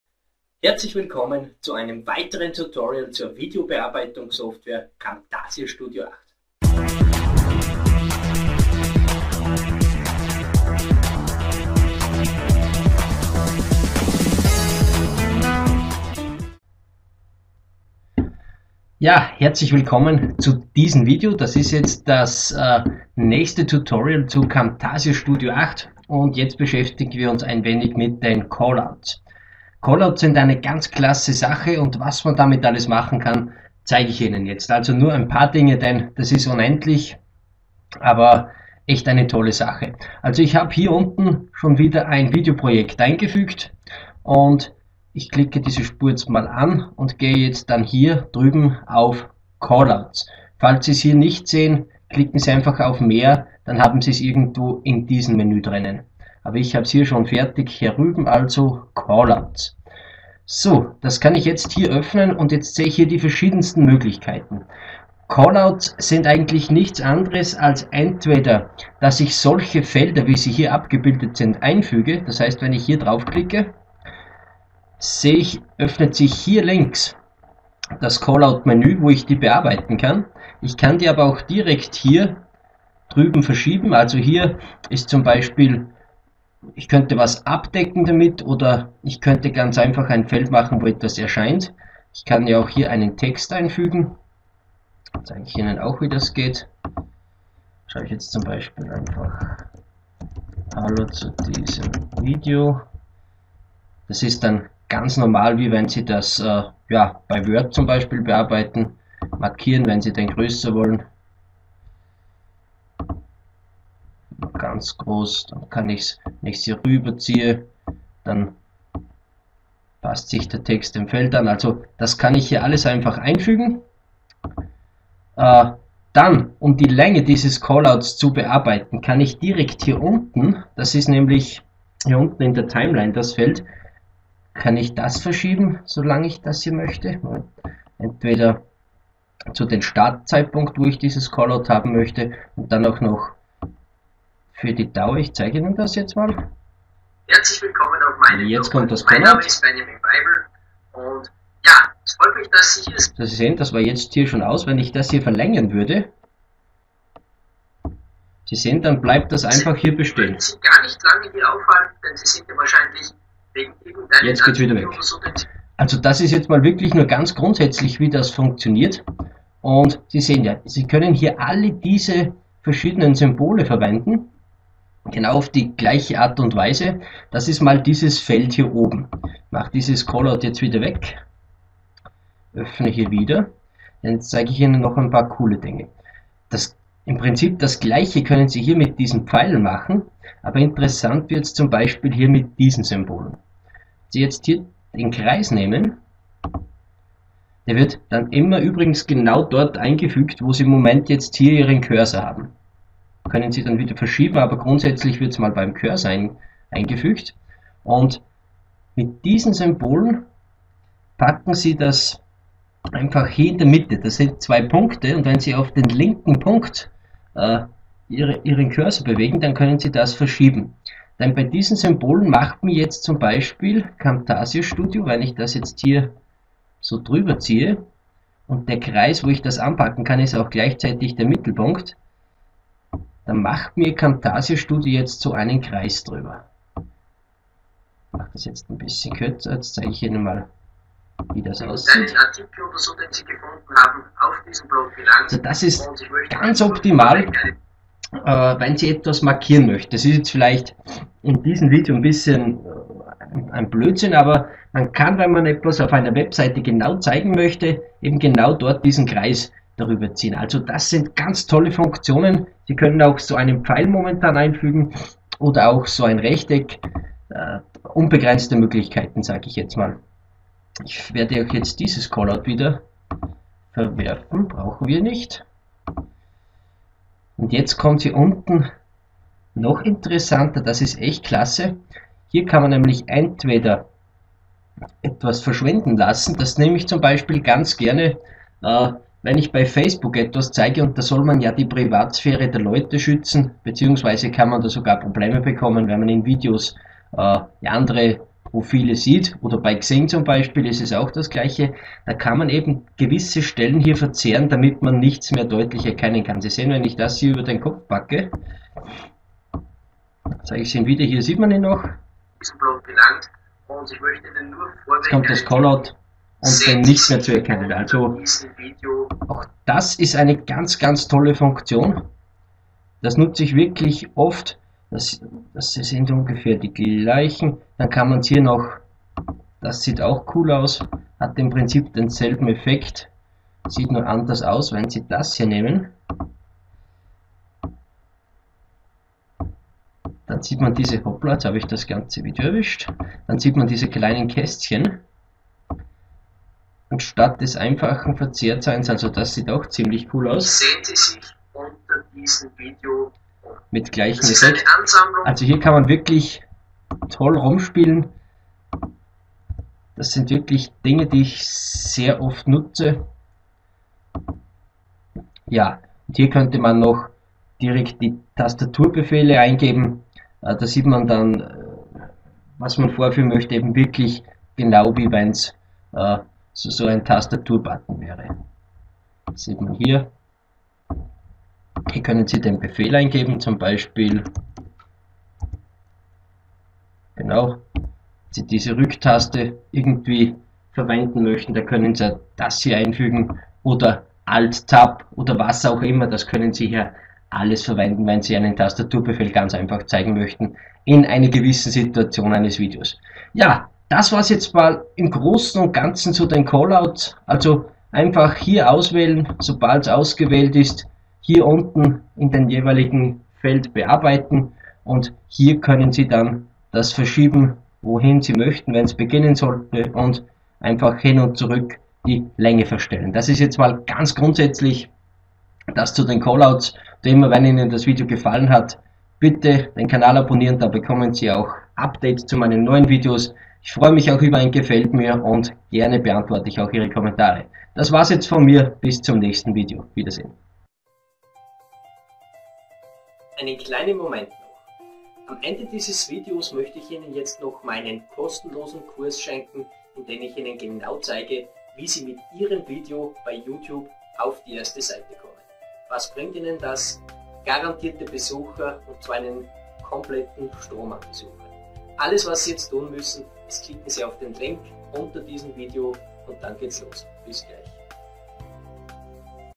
Videos erstellen mit Camtasia Studio 8 Die Callouts Tutorial Deutsch
Videos erstellen mit Camtasia Studio 8 Die Callouts Tutorial Deutsch. In diesem Video geht es um die Callouts.

Mit den Callouts bei Camtasia Studio 8 können Sie tolle Felder einfügen, Sachen abdecken oder auch besondere Bereiche einkreisen oder hervorheben.

Auch hier sind den Möglichkeiten keine Grenzen gesetzt. Ich nutze die Callouts von Camtasia Studio 8 eigentlich bei jedem Video. Sie sind für professionelle Bildschirmpräsentationen ein Must have.